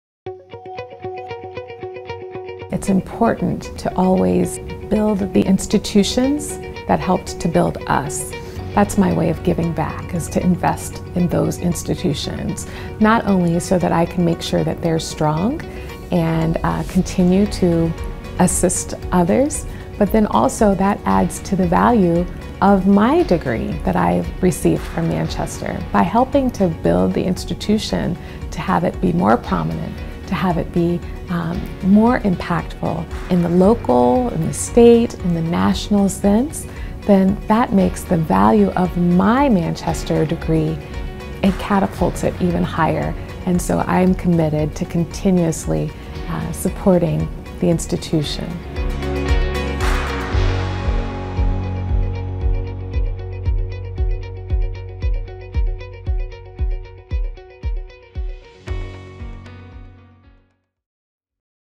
Why I Give to Manchester University – Alicia Roberts ’01 Baird
“Build the institution that helps to build us,” are words Alicia Roberts ’01 Baird lives by. She believes that giving to Manchester University not only strengthens the institution, but adds to the value of her own degree. The world needs more Manchester graduates.